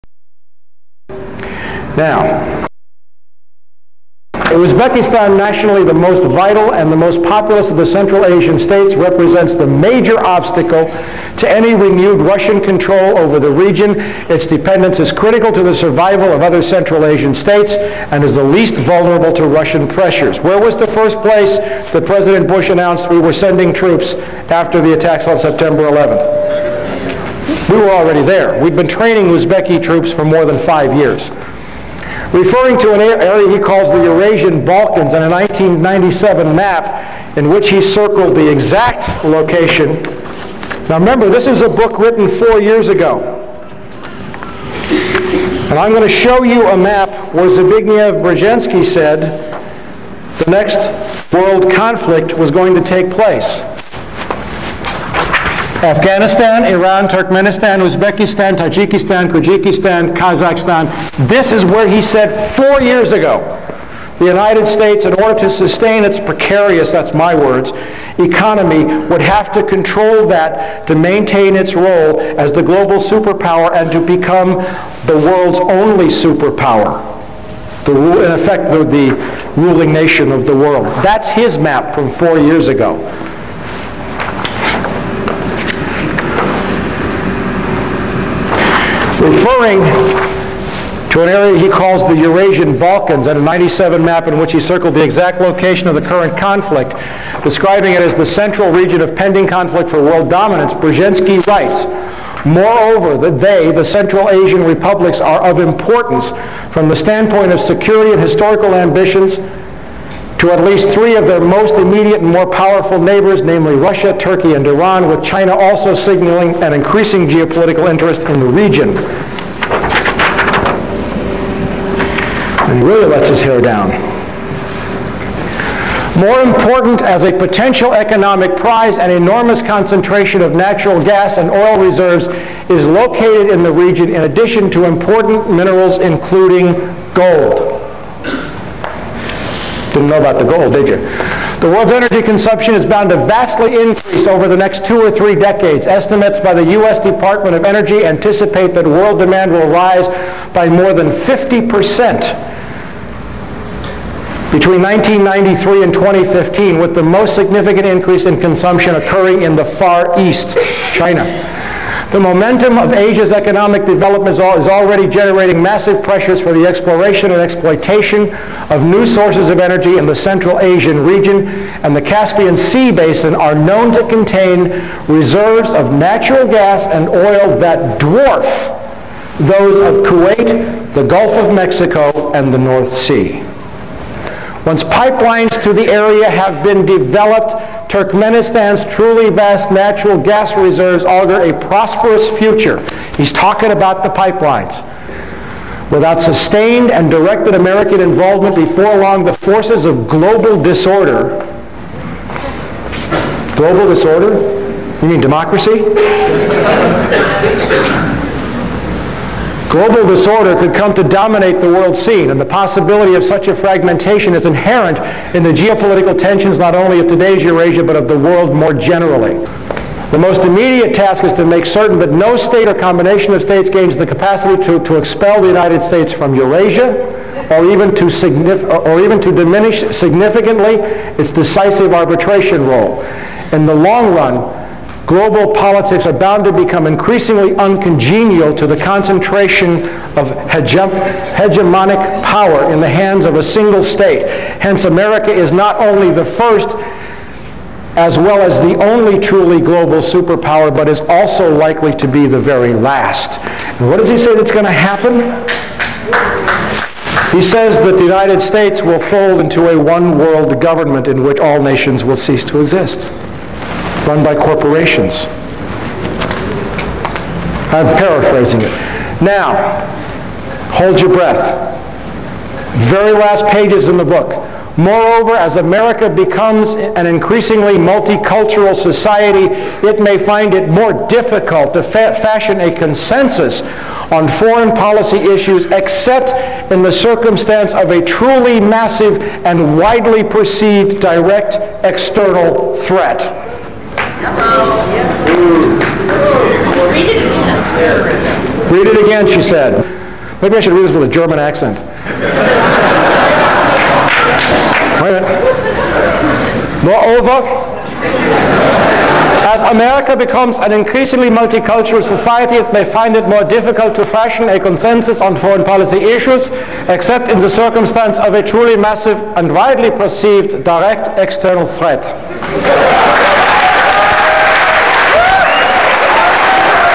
Z Big part 3 of 3
This is Mike Ruppert talking about the book "The Grand Chessboard: American Primacy and Its Geostrategic Imperatives" by Zbigniew Brzezinski